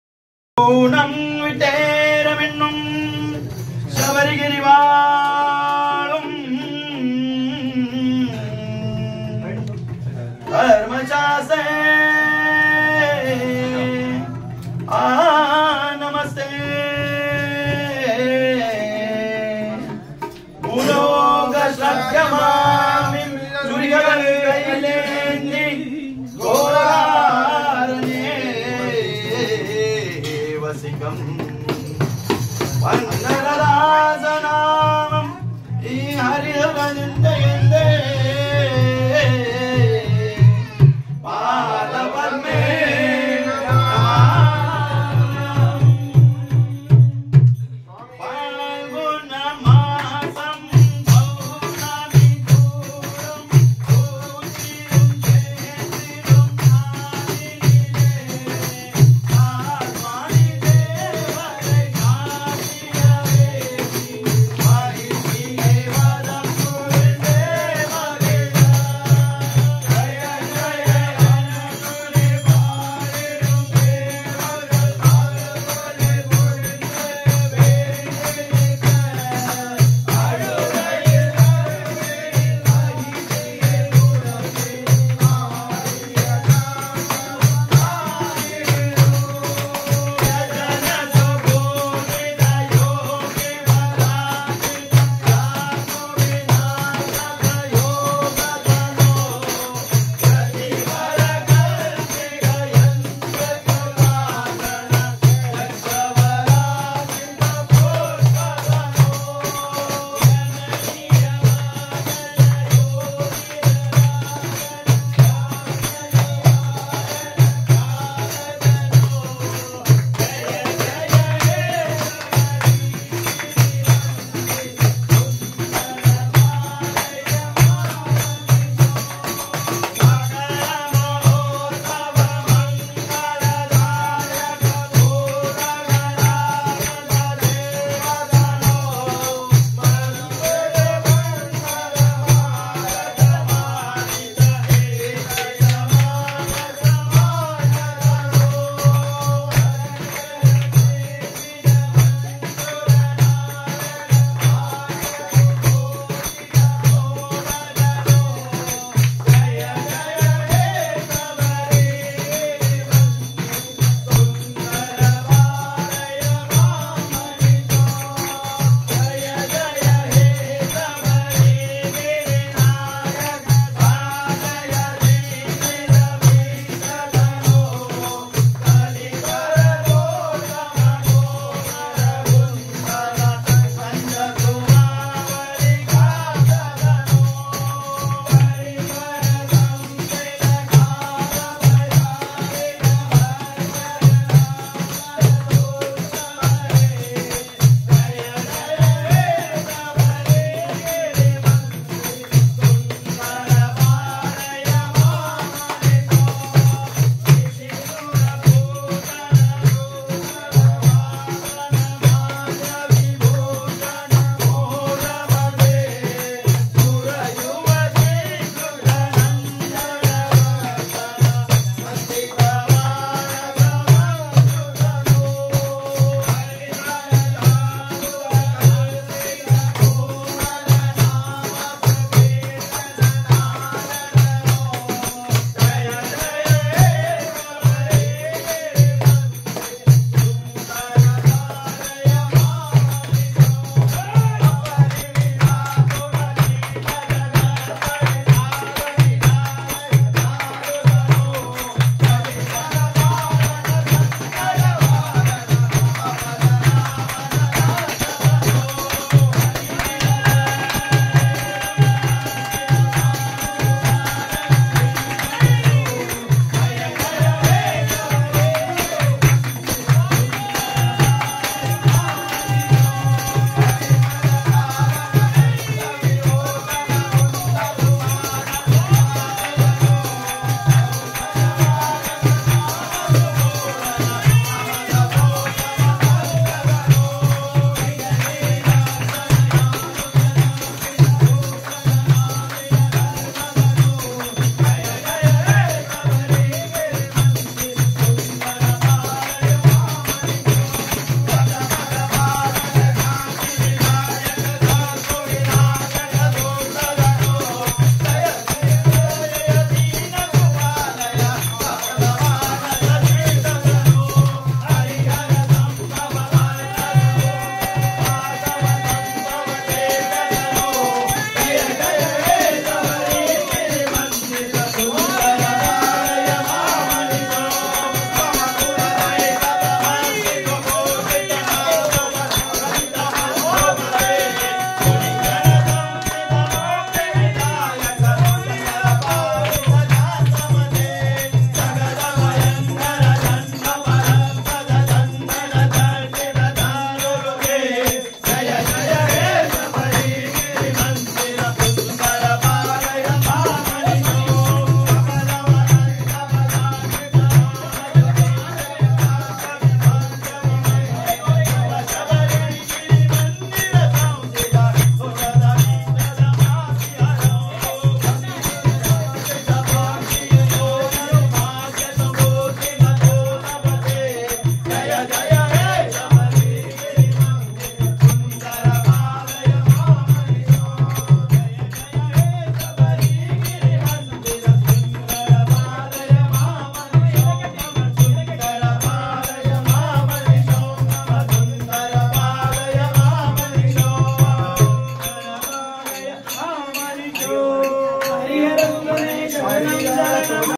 SABS 2023: SANTOSH AND SWAPNIL KHADYE SWAMI HOUSE SASTHA PREETHI POOJA HELD ON 29-10-2023.
Swamiye Saranam Ayyappa|||

With Eldery and our guruswamy blessing Santosh and Swapnil khadye Swami House Sastha Preethi pooja Held on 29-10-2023. SABS 2023 Sastha preethi pooja was conducted with full celebration with Poorna Kumbha Mariyadha of our'Alangara Priyan' Sri. Satish Naidu Swamy followed by Ashtothram pooja,Maladharanam of Aradhya Chitre, Swami Ayyappa Bhajanam (A Special BHAJAN SONG SUNG BY Saroja Mami and OUR GURUSWAMY CHIDAMBRAM Swami) and ended with Anna Dhanam. Please watch it exclusively on our sangam Youtube Channel 'SABS: Swami Ayyappa Bakth Sangham'.

Swamiye Saranam Ayyappa|||